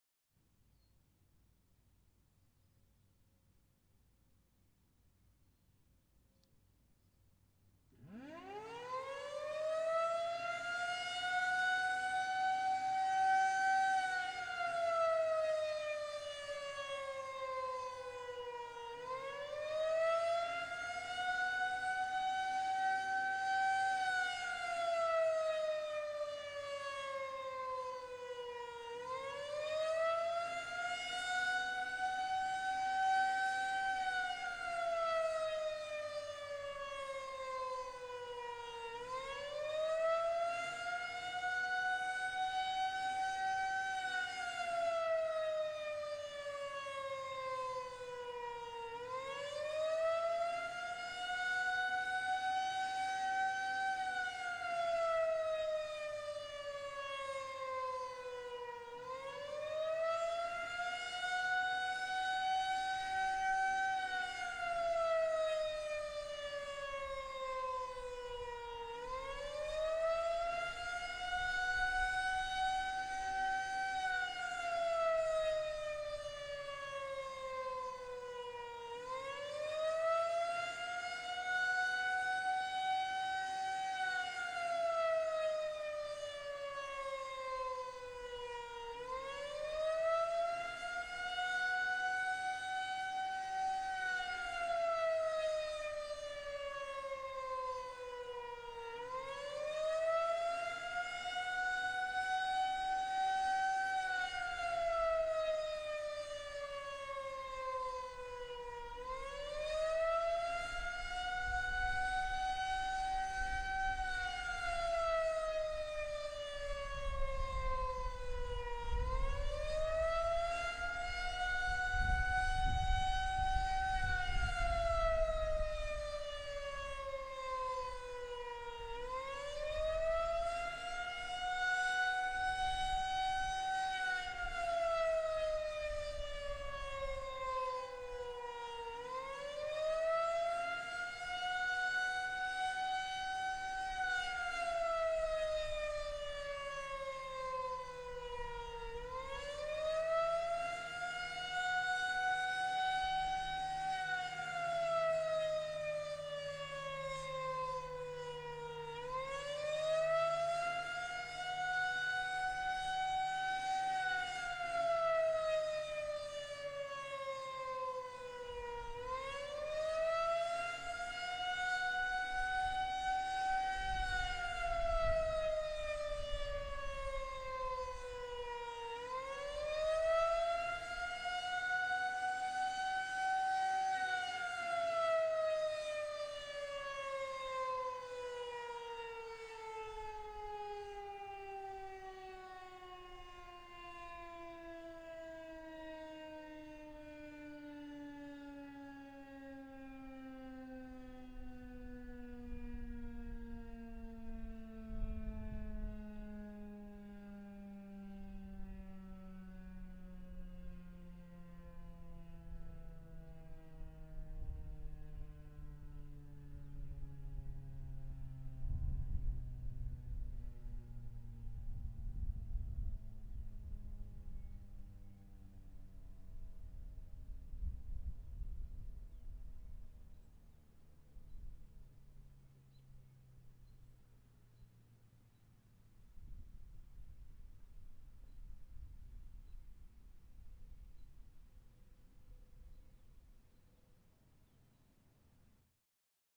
(Broken Rotator) FS 2001-130 Siren Test | Full Attack | Batesville, IN
**Recorded 6/22/24**

Decided to head over to Batesville with Benicio and Dominic, to film this 2001-130 in Bateville with Andrew. As you notice, the rotator is broken since the last time it was filmed, hopefully it'll be fixed soon. I ended up re-framing the shot in post. This unit was loud and punchy, although I have heard slightly louder '130's.

*Additional Info*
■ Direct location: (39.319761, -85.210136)
■ Testing: Every Saturday @ noon
■ Recording distance: 127ft
■ Main shot: Panasonic Lumix DC-FZ80
■ Second shot: Sony CyberShot DSC-HX20V
■ Audio: Zoom H1N Portable Audio Recorder
■ Channels present: @Creeperfan12, @CentralOHSirens, @SuperSirex127